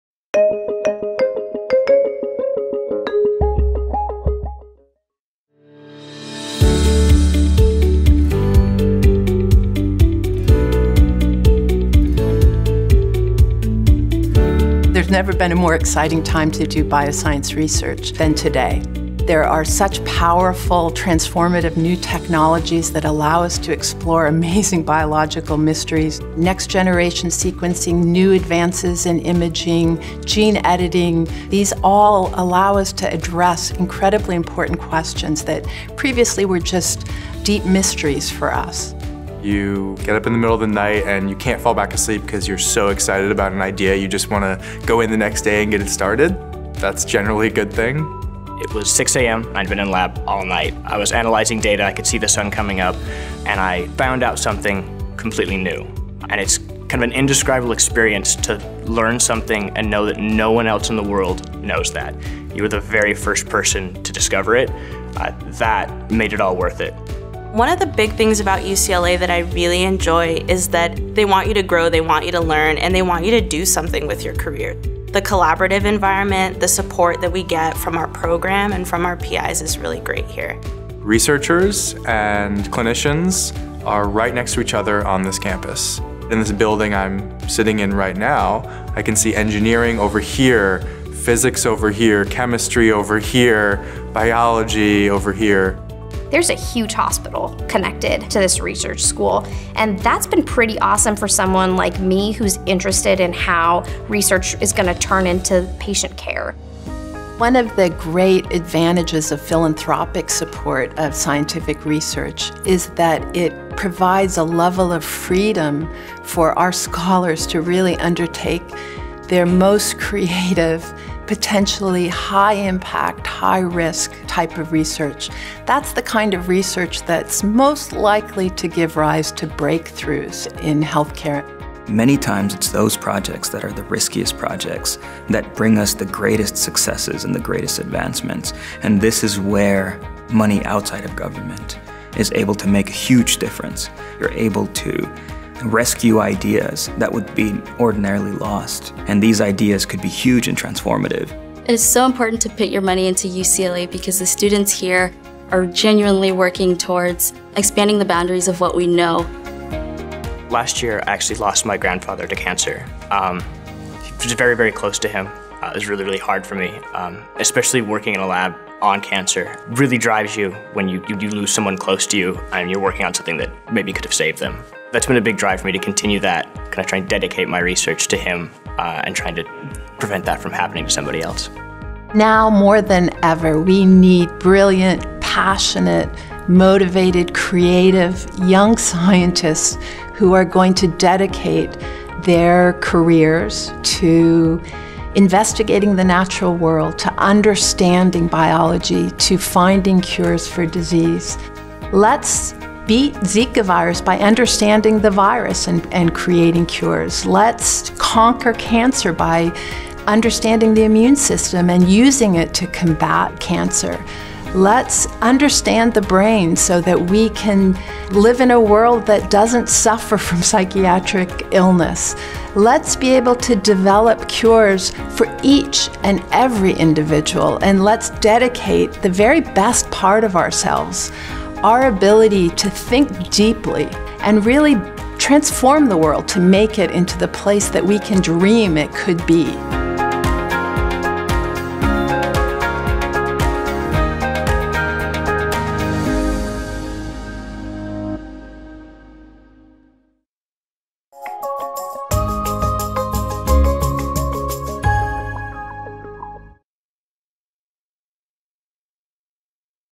The Value of Basic Science | David Geffen School of Medicine at UCLA
New advances allow us to address incredibly important questions that were previously complex mysteries. Kelsey Martin, M.D., Ph.D., Dean at the David Geffen School of Medicine at UCLA talks about the importance of scientific discovery and explains how she envisions we can 'dedicate the very best part of ourselves - our ability to think deeply and to transform the world into a place that we dream it could be.'